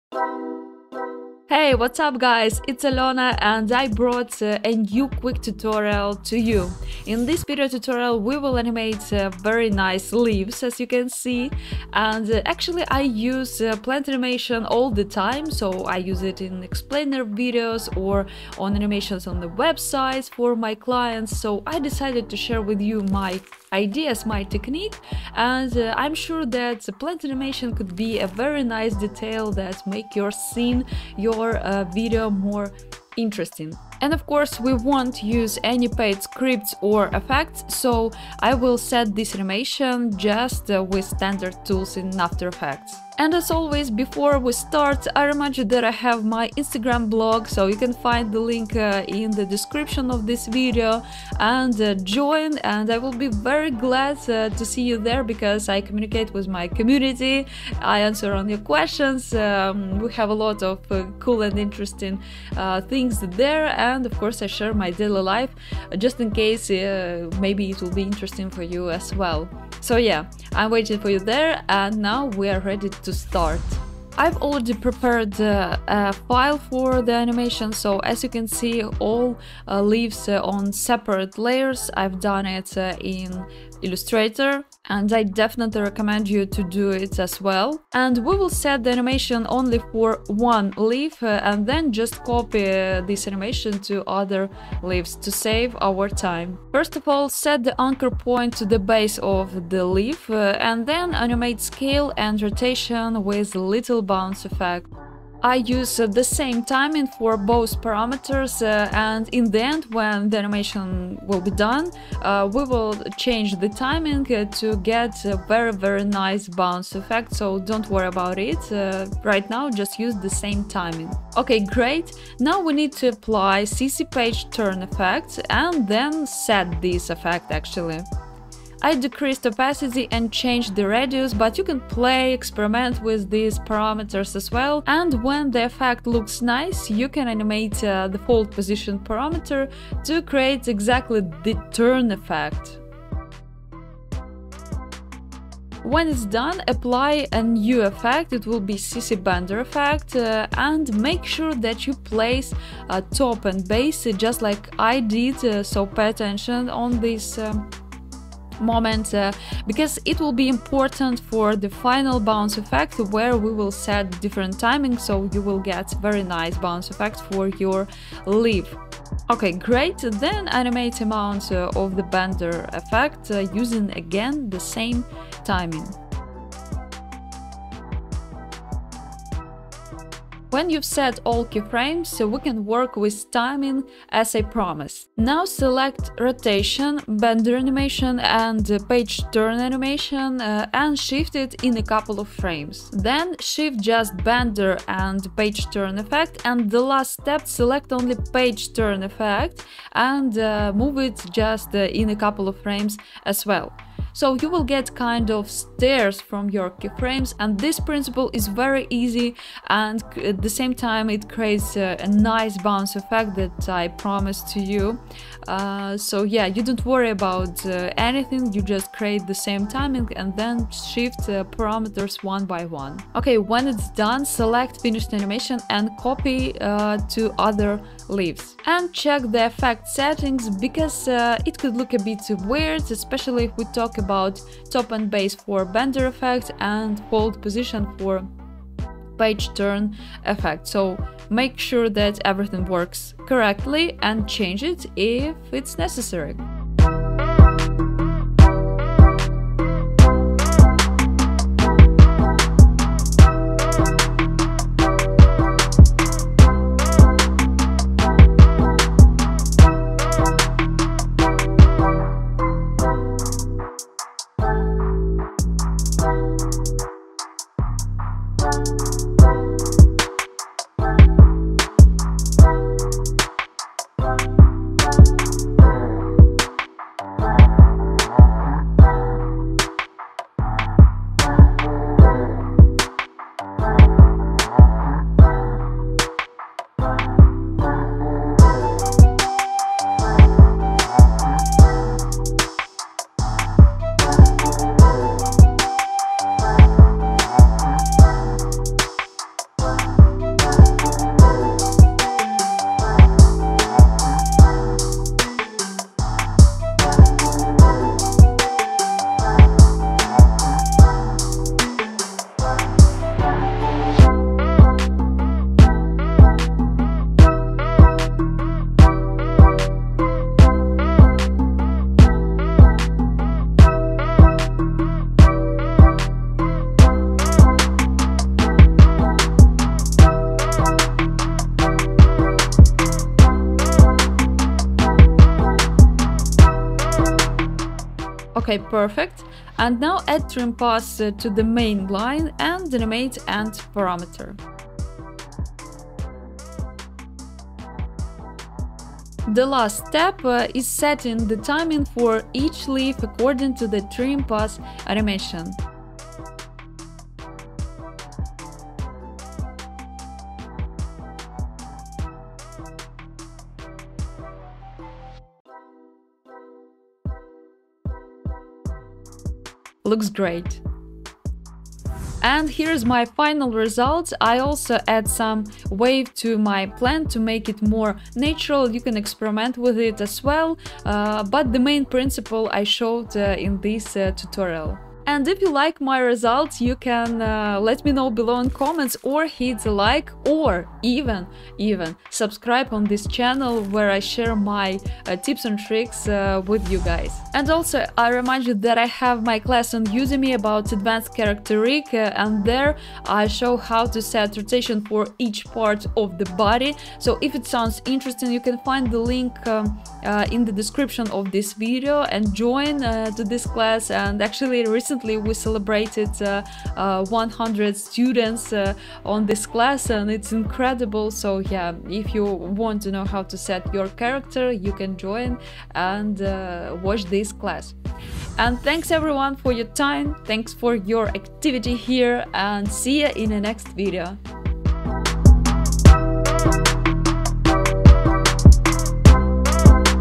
HOW TO ANIMATE LEAVES IN AFTER EFFECTS. NO PLUG-INS!
Today we will animate a nice plant using just standard tools in After Effects.